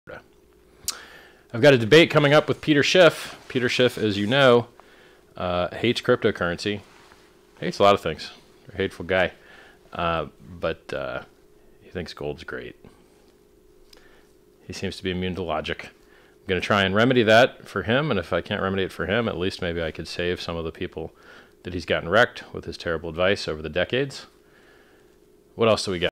RICHARD HEART VS PETER SCHIFF @ 10PM GMT 23/02
Just rich talking about the debate.